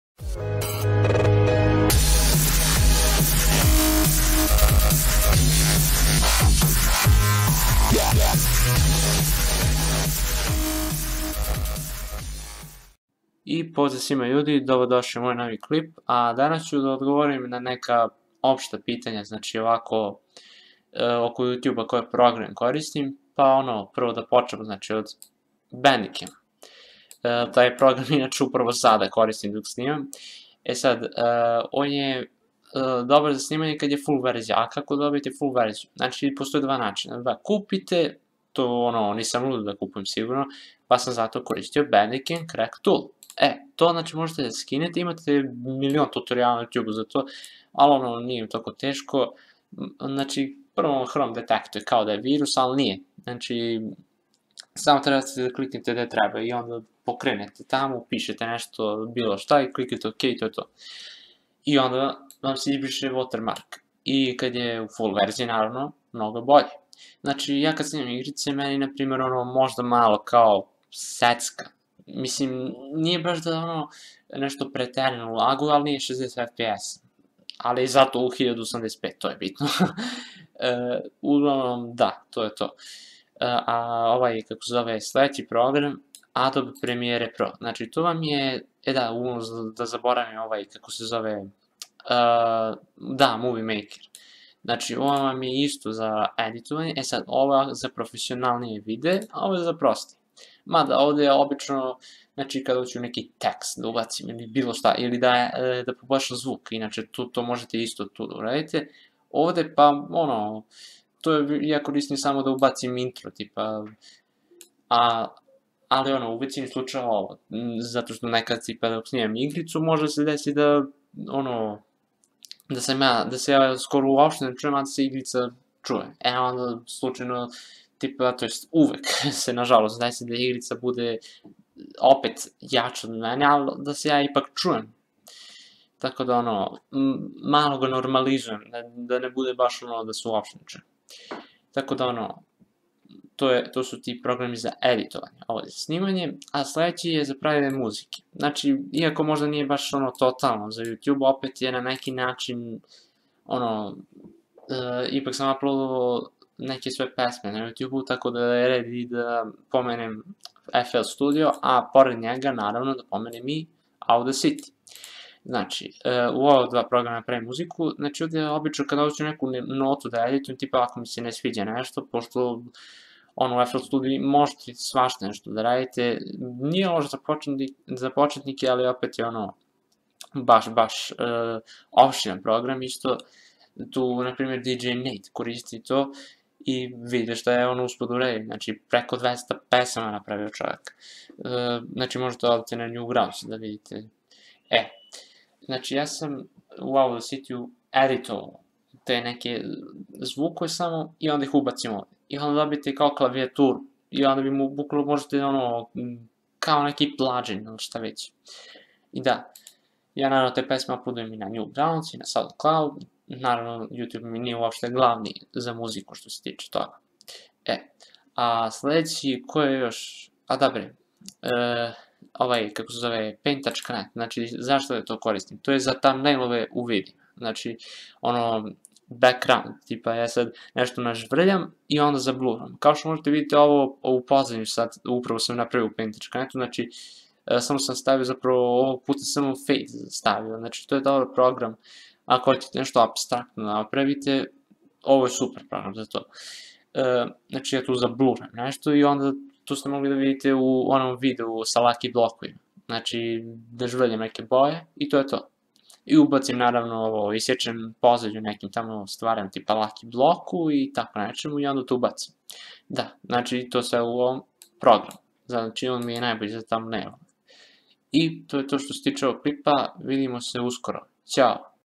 Koje programe koristim za YT?
Odgovori na neka osnovna pitanja.
Computer:
Graphic card: Radeon RX 550 2GB GDDR5 DirectX 12
Processor: Intel core i5-2400 quad 3.1 GHz
RAM: 8GB
OS: Windows 10 64-bit